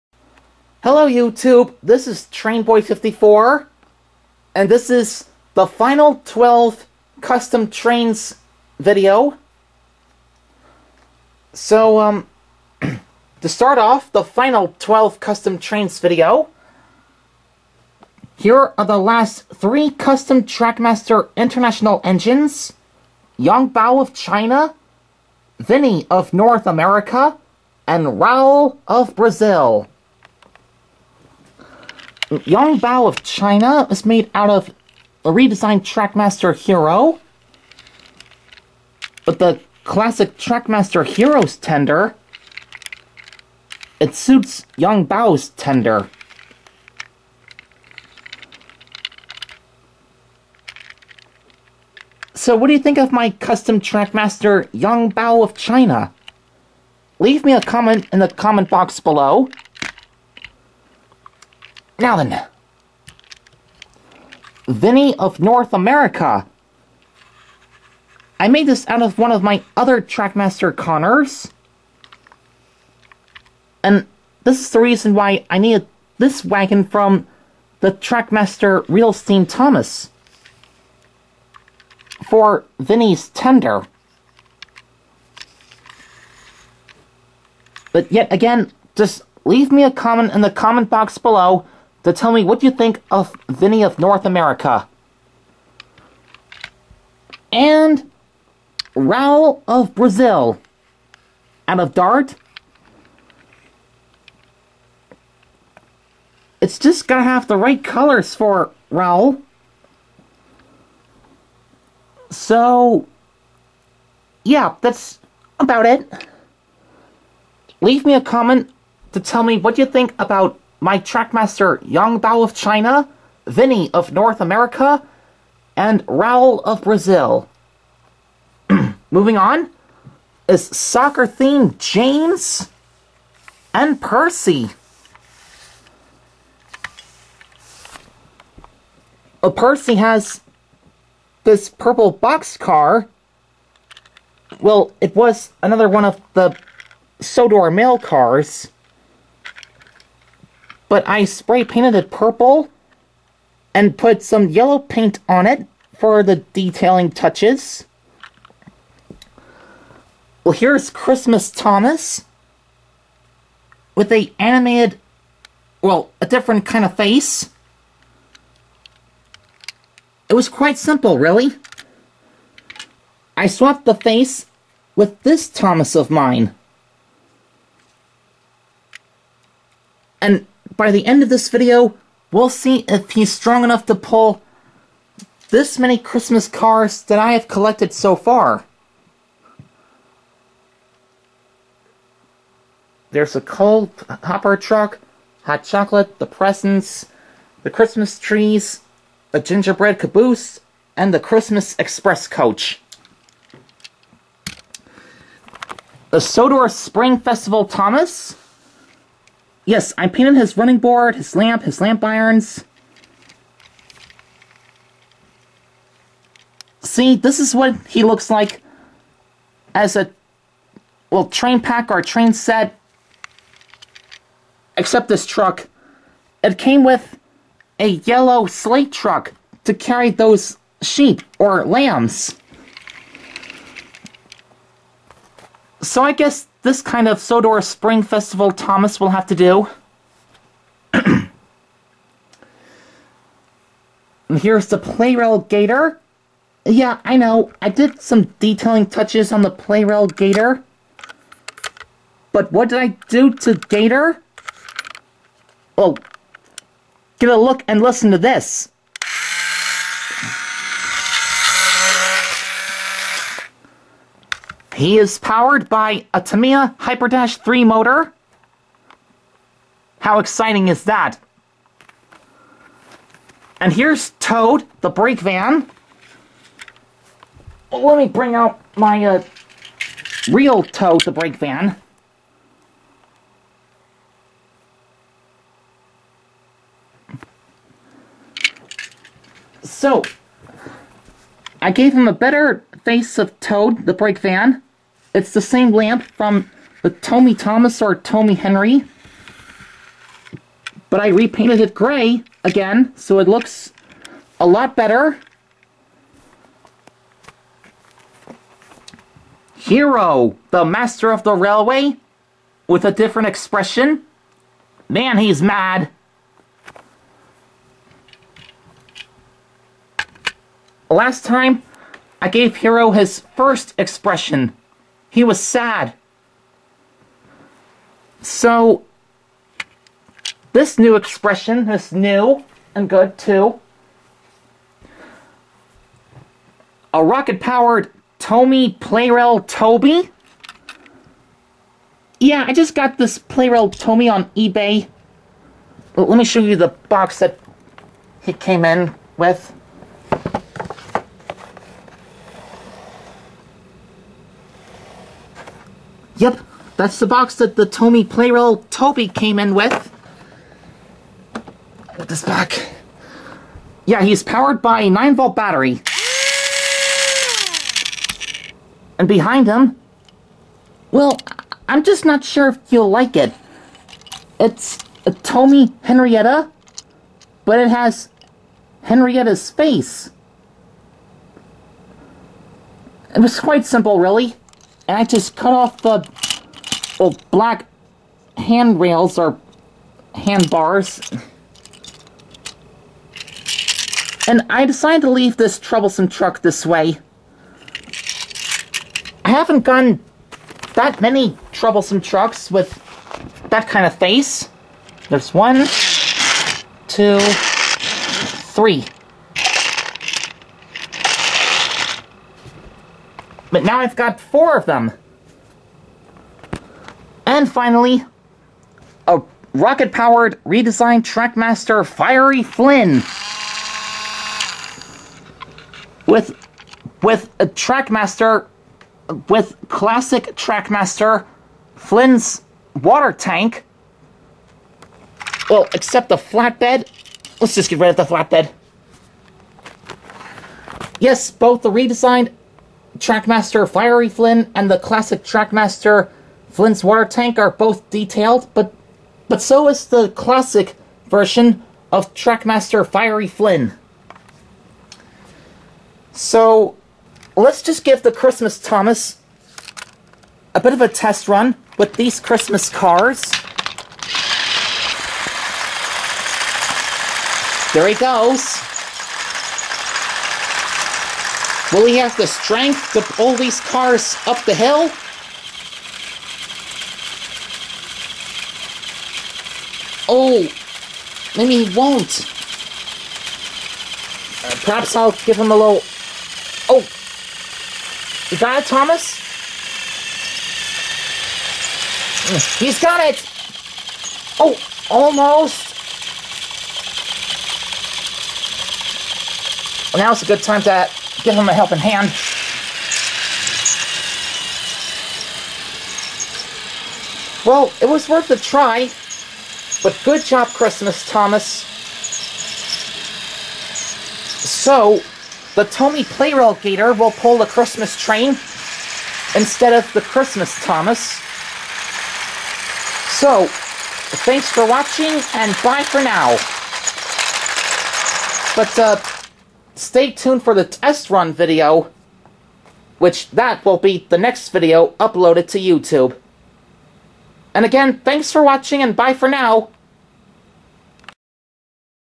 12 Custom Trackmaster Thomas Trains10 The Last 12 Custom Trains
I'm sorry to say this: But This is the last 12 Custom Trackmaster Thomas Trains Video. I'll think of how many more custom Trackmaster Thomas Trains I'll make. In the mean time, I want you folks to write a comment in the comment box to tell me which of the last 12 custom trains is your favorite. The next video is their test runs and it will put up on youtube soon.